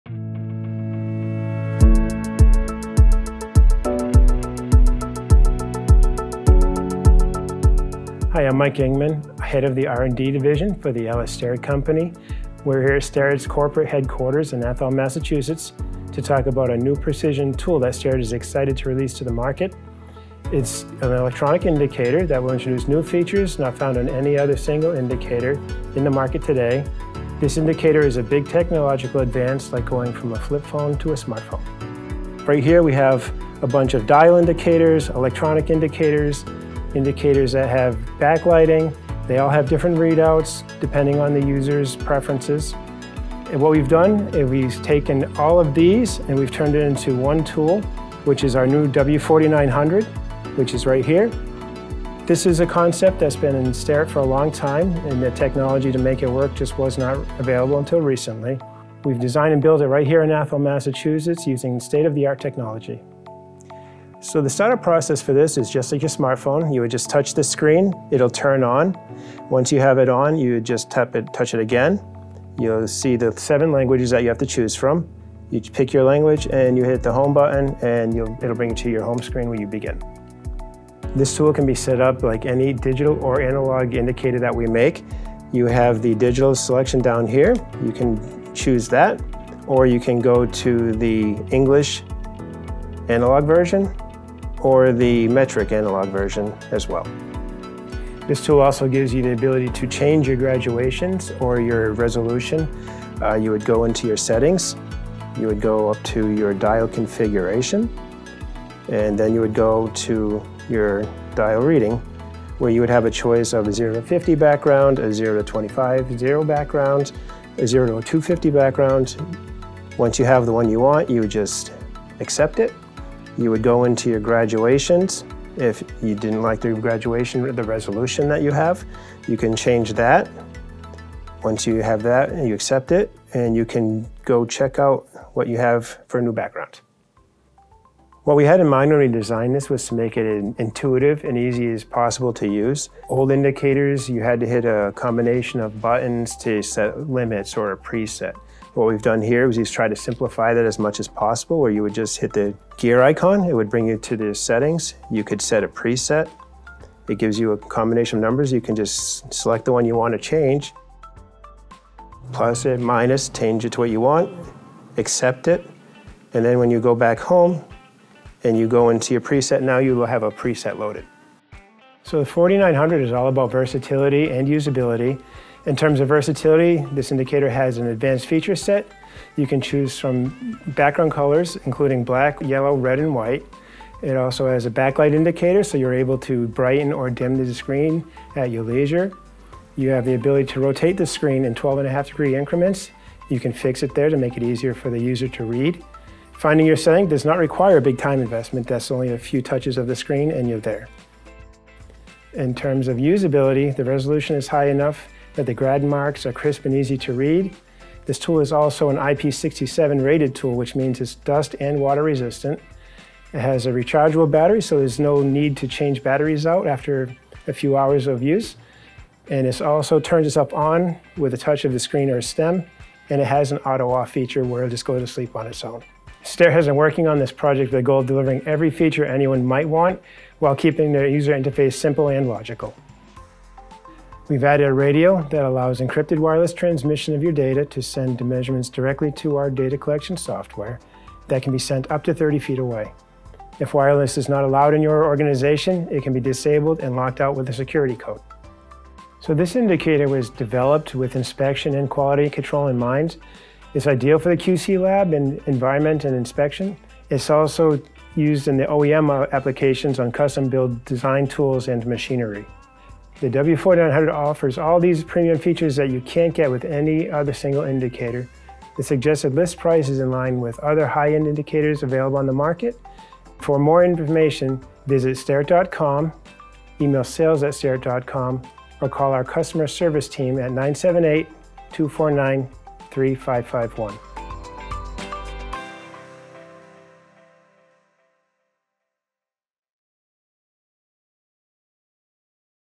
Introducing W4900 TOUCHSCREEN ELECTRONIC INDICATORS (WITH OUTPUT)
The new Starrett W4900 Electronic Indicator combines innovation, high versatility and ease-of-use with unparalleled ease of settings navigation. Built for quality labs, inspection areas and in-process applications where access is not always available. This high-end, IP67 indicator can also be used in OEM applications on custom build and design tools and machinery.